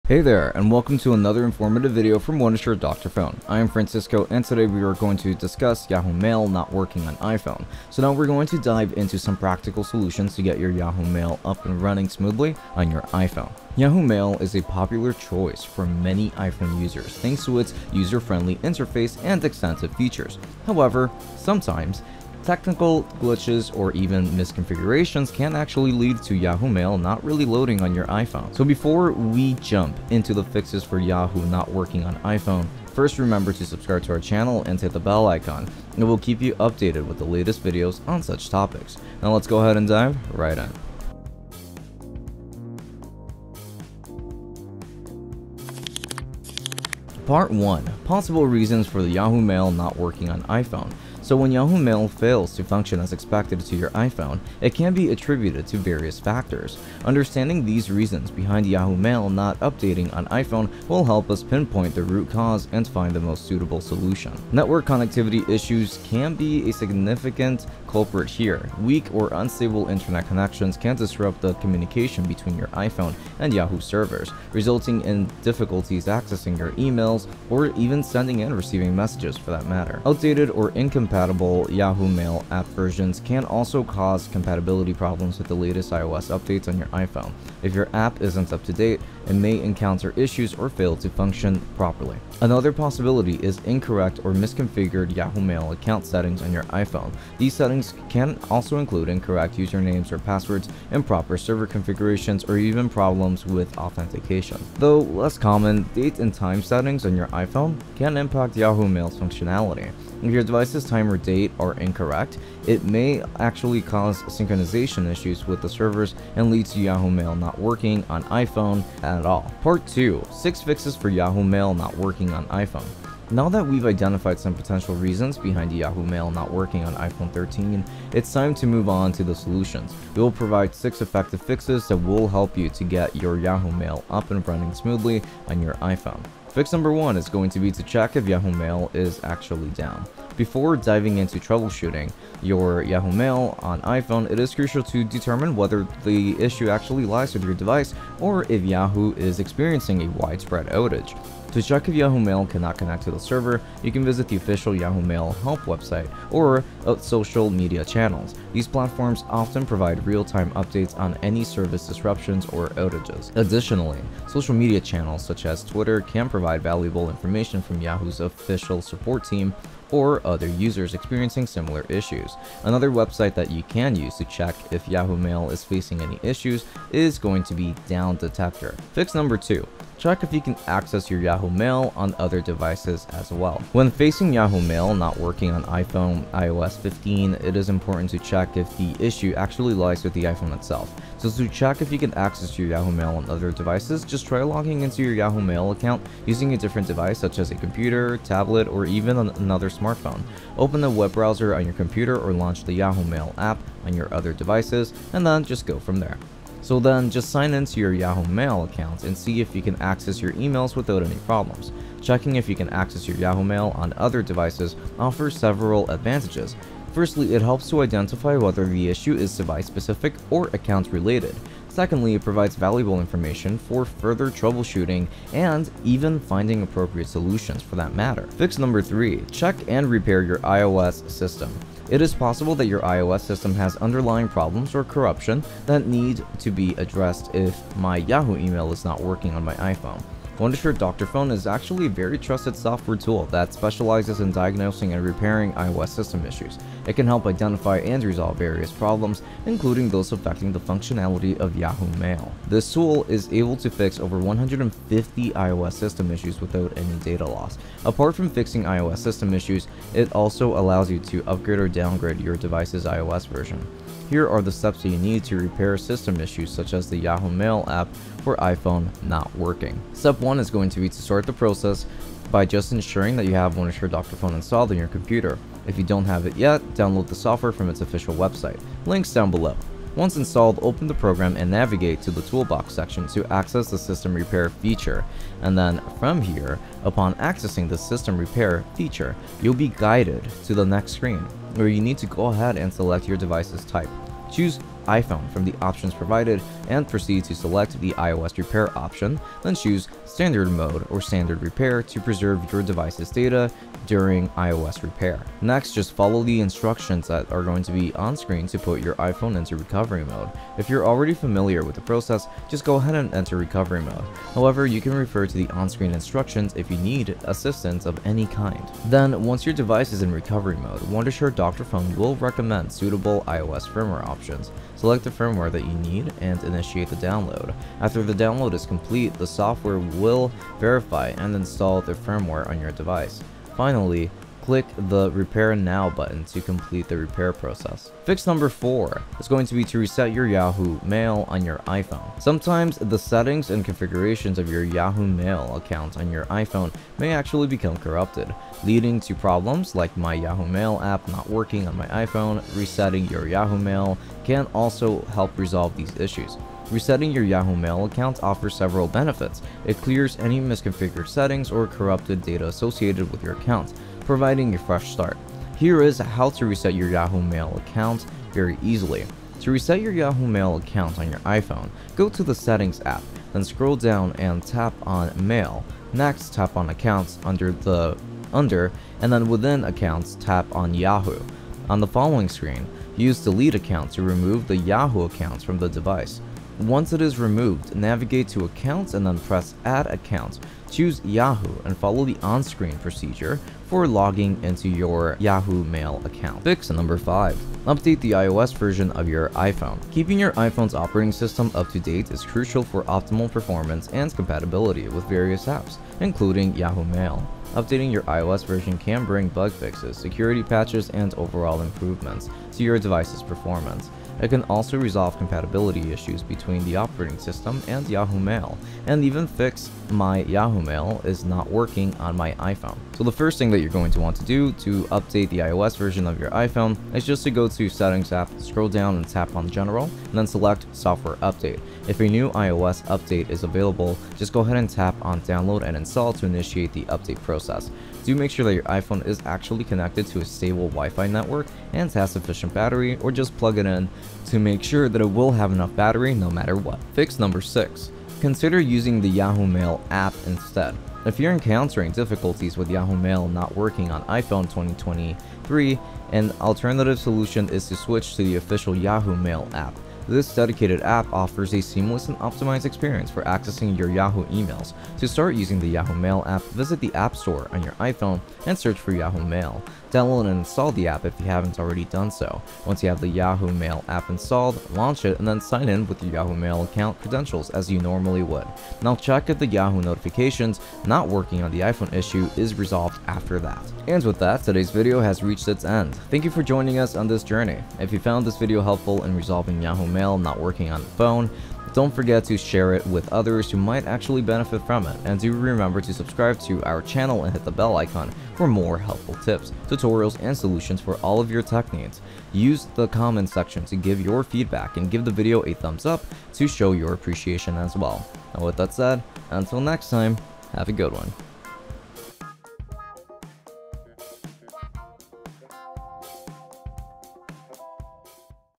6 Fixes For Yahoo Mail Not Working on iPhone
Is your Yahoo Mail acting up on your iPhone? Frustrating, isn't it? Don't worry, we've got your back! In this comprehensive guide, we'll walk you through 6 effective fixes to get your Yahoo Mail back on track and running smoothly on your iPhone.
🔧 Whether you're facing issues with login errors, syncing problems, or simply not receiving emails, our step-by-step solutions will help you troubleshoot and resolve the problem in no time.

Here's a sneak peek of what you'll learn in this video:
🔹 0:00 Introduction
🔹 0:47 Possible Reasons for the Yahoo Mail Not Working on iPhone
🔹 1:06 Network connectivity
🔹 1:21 Outdated App
🔹 1:33 Incorrect settings
🔹 1:46 Date and Time
🔹 1:59 6 Fixes for Yahoo Mail Not Working on iPhone
🔹 2:17 Fix 1: Check if Yahoo Mail is down
🔹 2:59 Fix 2: Check if you can access your Yahoo Mail on other devices
🔹 3:49 Fix 3: Check and repair your iOS System
🔹 5:40 Fix 4: Reset your Yahoo Mail on your iPhone
🔹 6:37 Fix 5: Check the IMAP Settings on your iPhone
🔹 7:27 Fix 6: Consider using the Yahoo Mail app instead
🔹 8:04 Conclusion

Don't let Yahoo Mail issues ruin your productivity and communication flow. Follow our easy-to-follow fixes and enjoy uninterrupted access to your emails on your iPhone. Let's get your Yahoo Mail back in action!